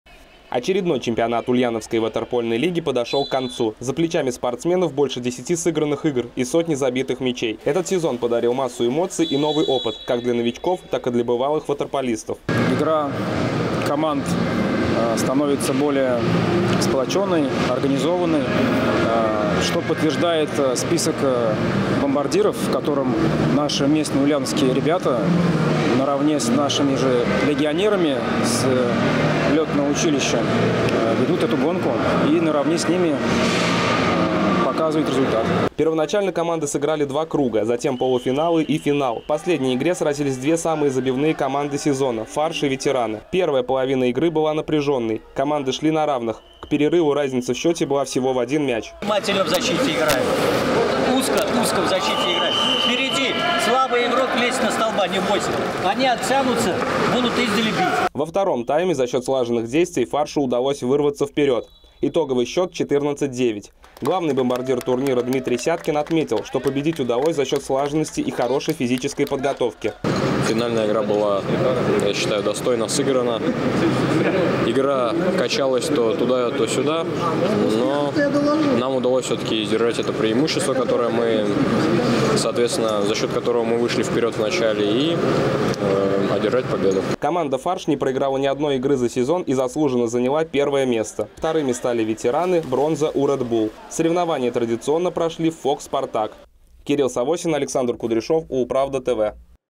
Победа за «Фаршем». В Ульяновске завершился чемпионат по мини водному поло
Яркий и непредсказуемый. Таким выдался чемпионат Ульяновской области по мини водному поло. В упорной борьбе четыре команды разыграли между собой награды.